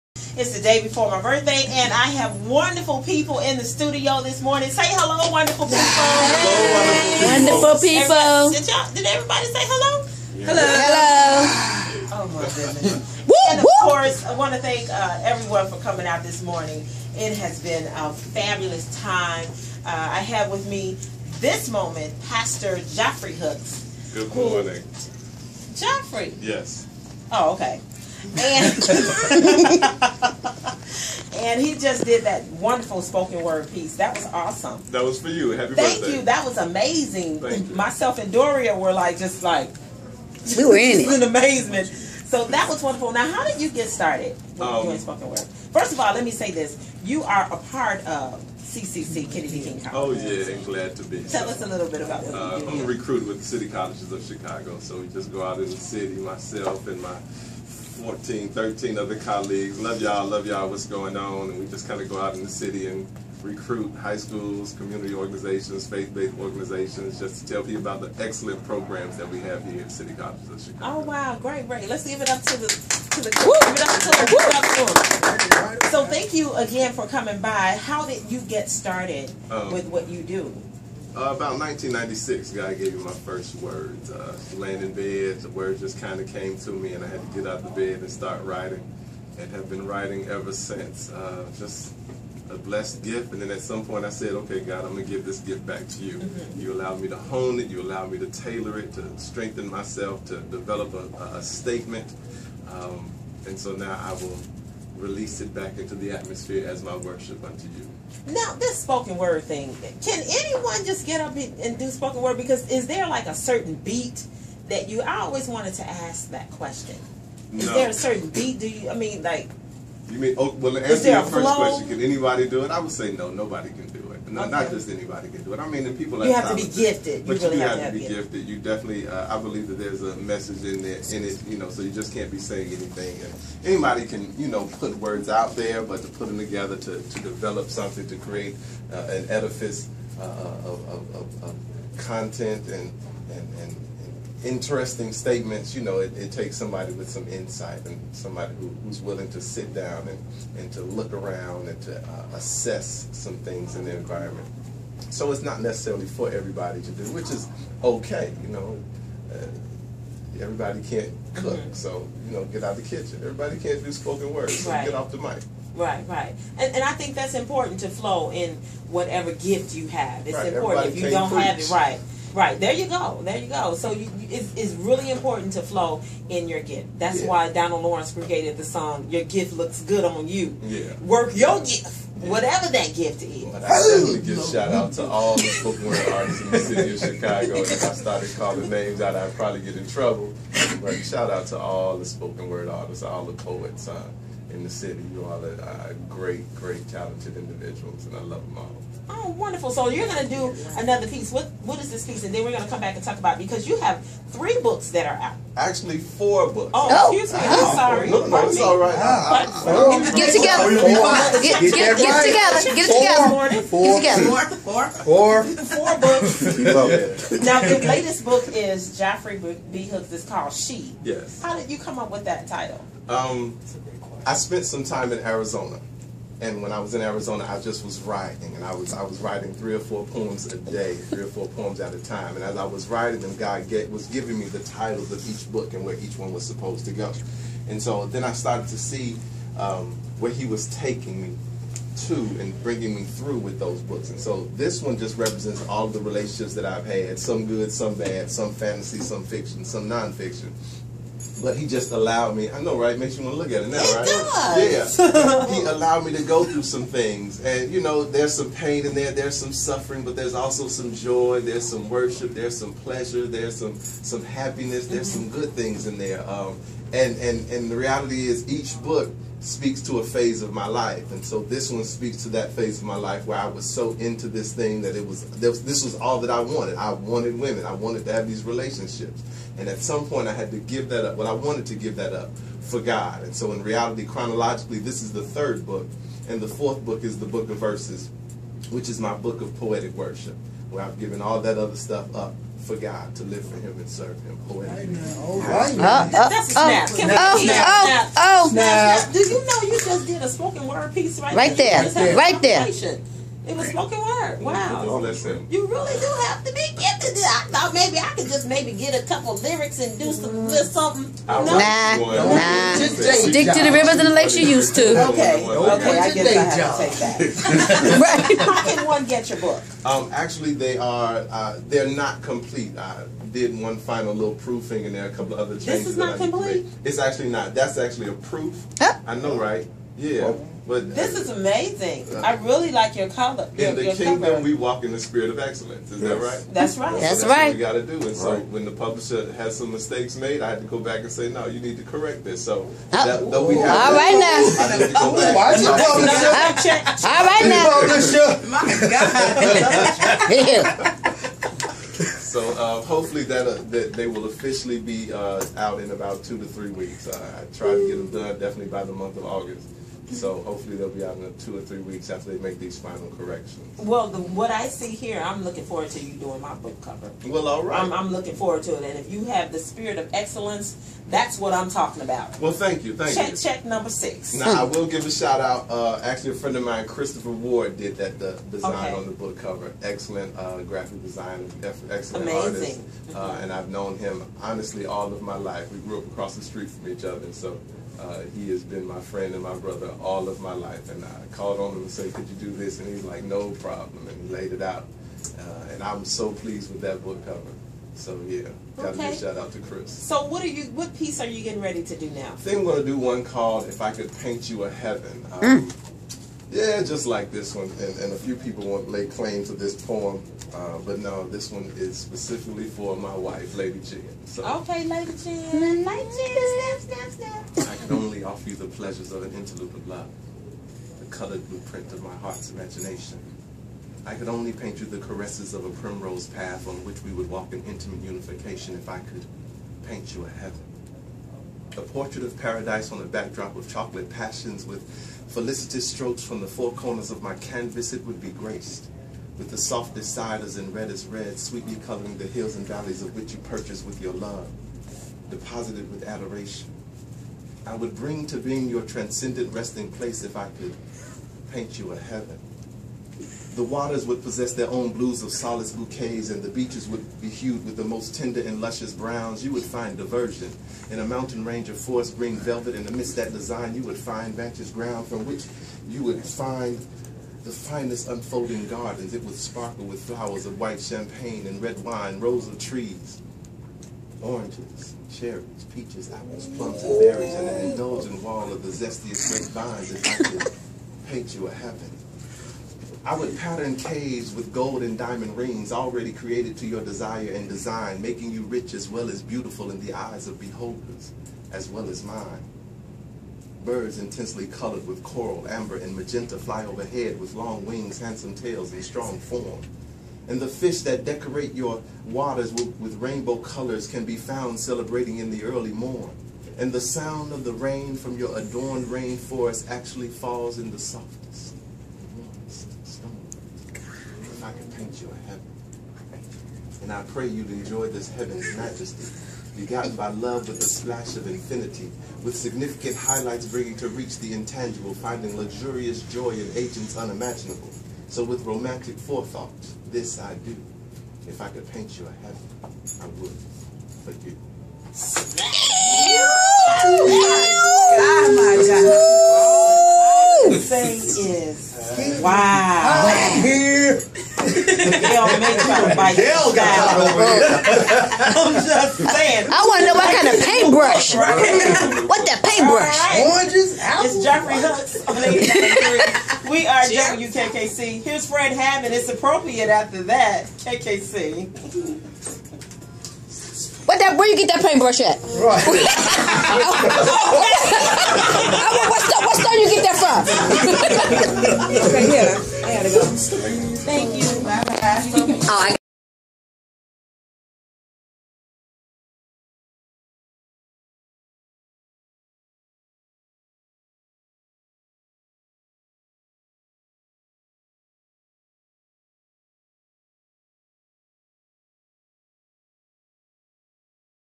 JOFFREY HOOKS at WKKC w/MARCELLA JONES---If I Could Paint You A Heaven
It was Marcella's birthday and I came by the radio station, 89.3 WKKC, to celebrate with her and the radio crew.  She did an impromptu interview, allowed me to promote my book a lil bit, and I did a poem for the show. (I actually did 2 poems, but the first one didn't get recorded...sorry guys)